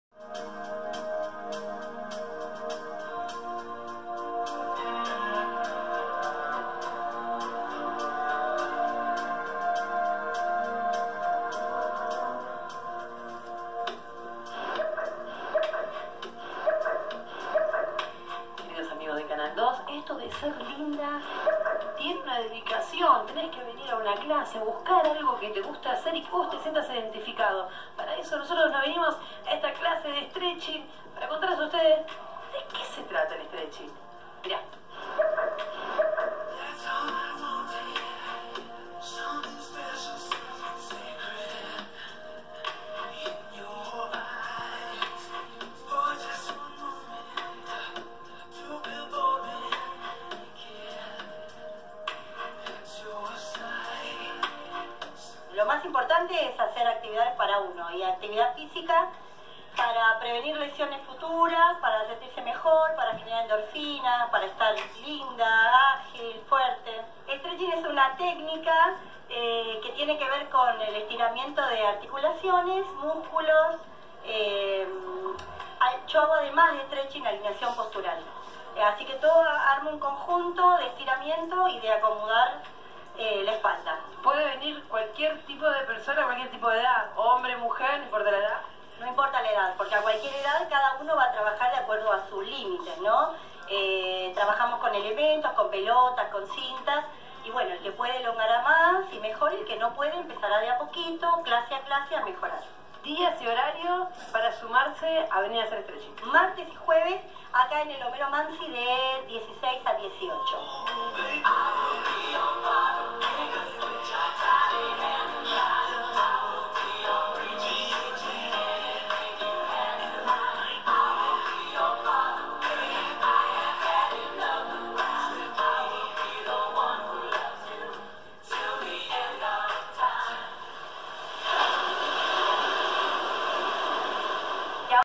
Streaching y Alineación Postural by Irene Steimbach Tarnovsky
En el Centro Cultural Homero Manzo de Villa Gesell - AEGENTINA
martes y jueves de 16 a 18 ha
También en el Centro Cultural Pipach de Villa Gesell - ARGENTINA
martes y jueves de 830 a 1030 AM
INSCRIBIRTE!!! te lo vas a perder ???